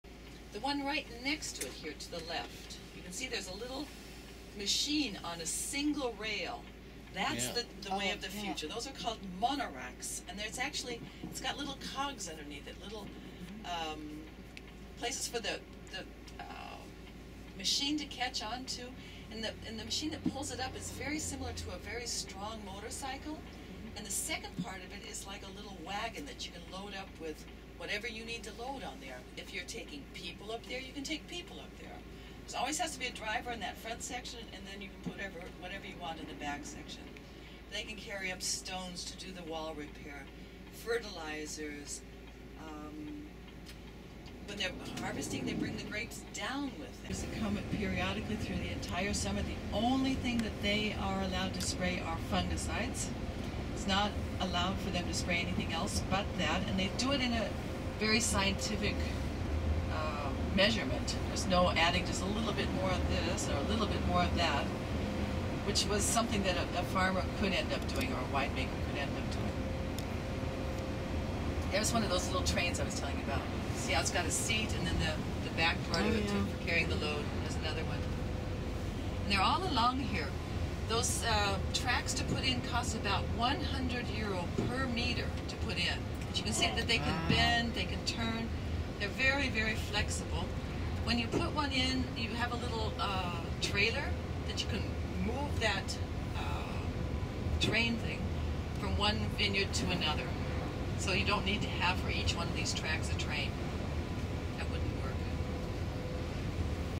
German mono rack wine cart train and tracks on steep Moselle River bank
This is a "mono rack" wine cart train and track in the Winninger-Uhlen vineyard on the banks of the Moselle River between the towns of Winningen and Kobern-Gondorf. The voice being heard is the Viking Cruises tour guide and the setting is an optional Moselle wine tour that we are on. The guide made a point of explaining that the track which this rig runs on costs 100 euros per meter. The date of the video is June 30, 2016.